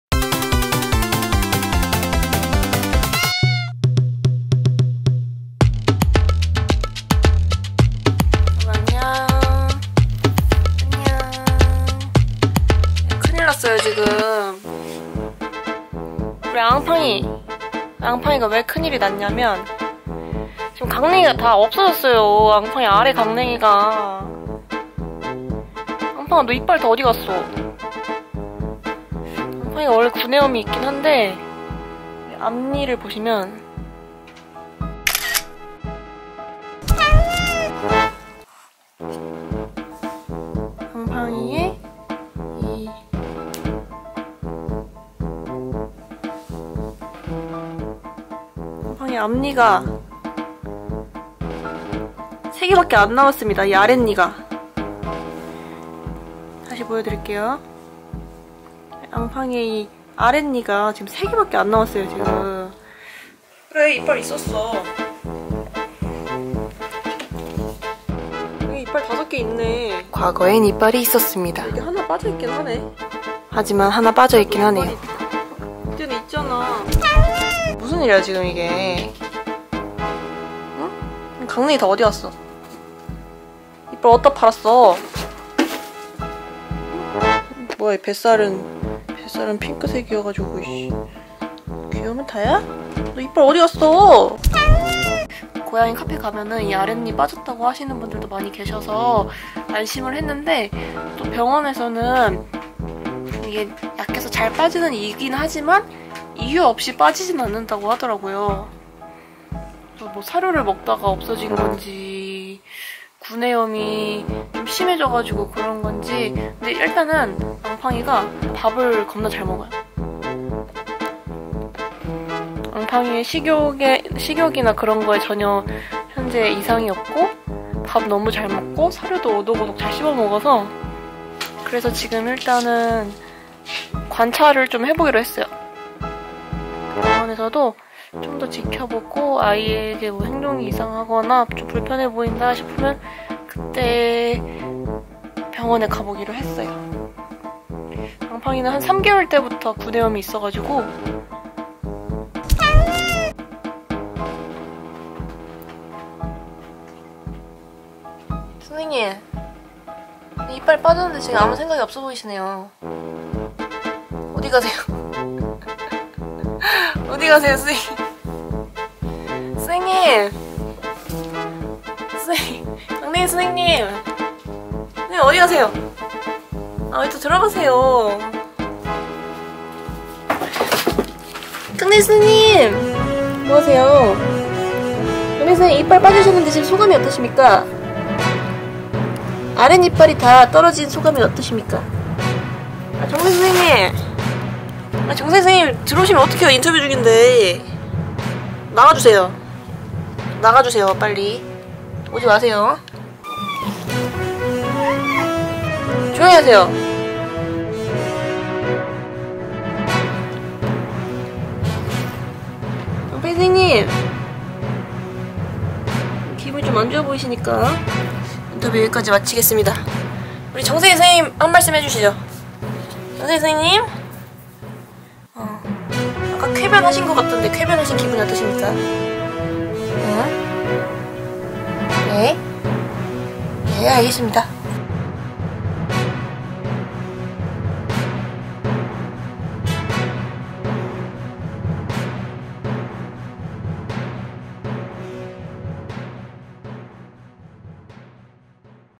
ENG) 잃어버린 강냉이를 찾아서ㅠㅠ 고양이 이빨 실종사건
강냉이는 어디에 ㅠㅠㅠㅠ

[고양이들 소개 동영상]

- ✉️팬 사서함: 수원시 영통구 삼성로 11, 208동 1602호
- 📼방송 LIVE : 매주 금요일 밤